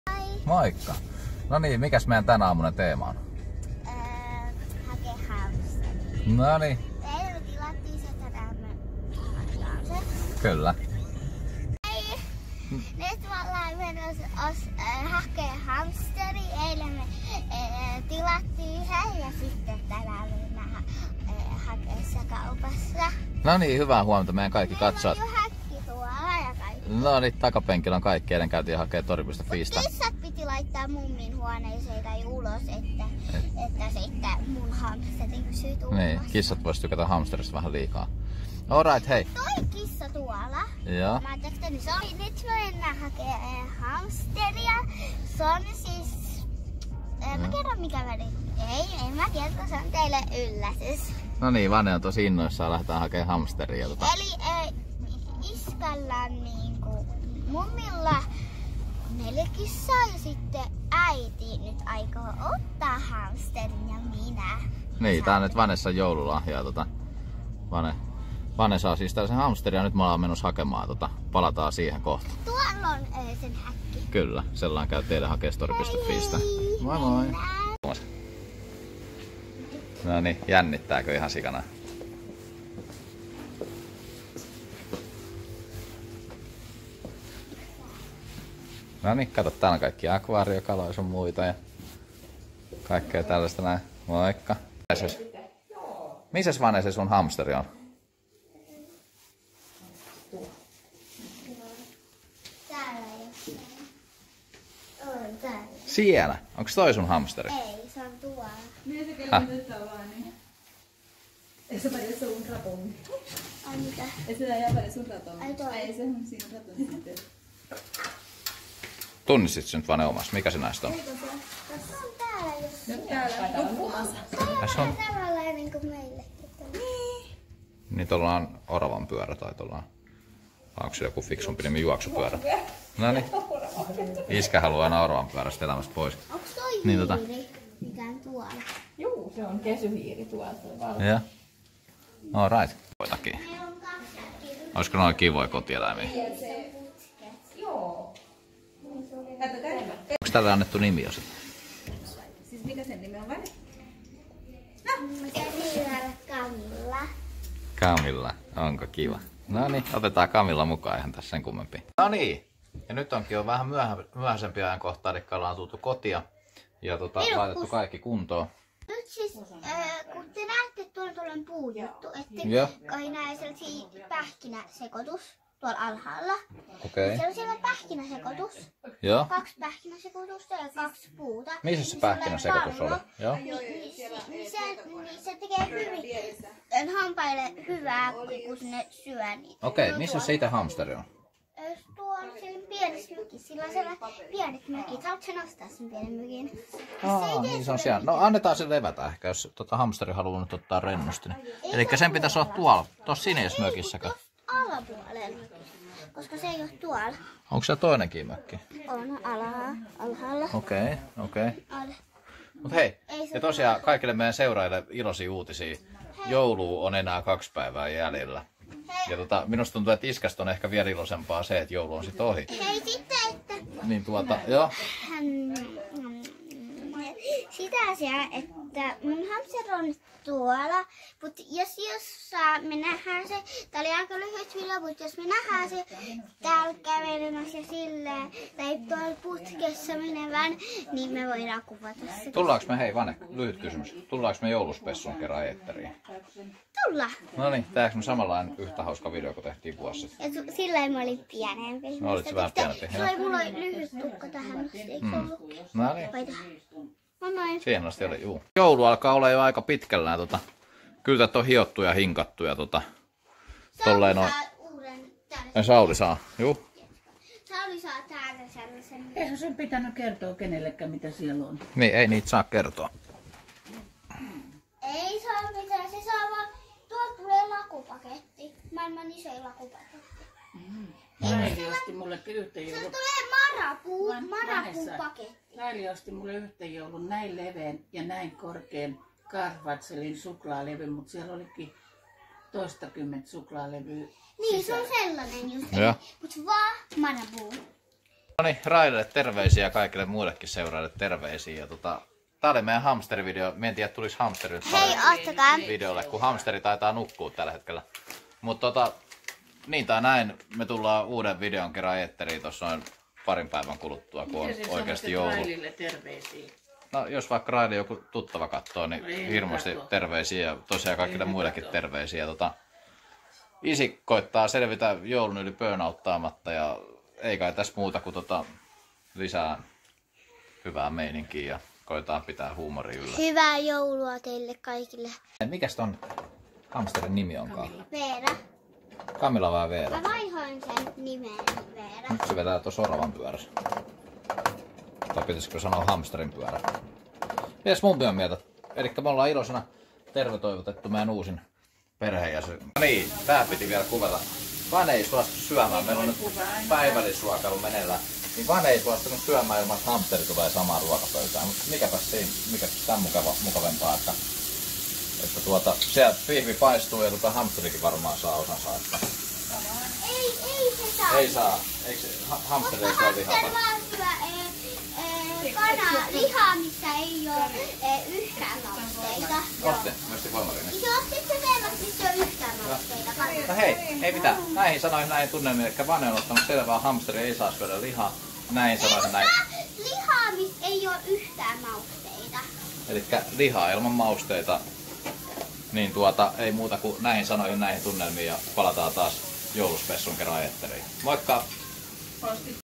Vanessa sai hamsterin
Käytiin eläinkaupassa ja ostettiin hamsteri.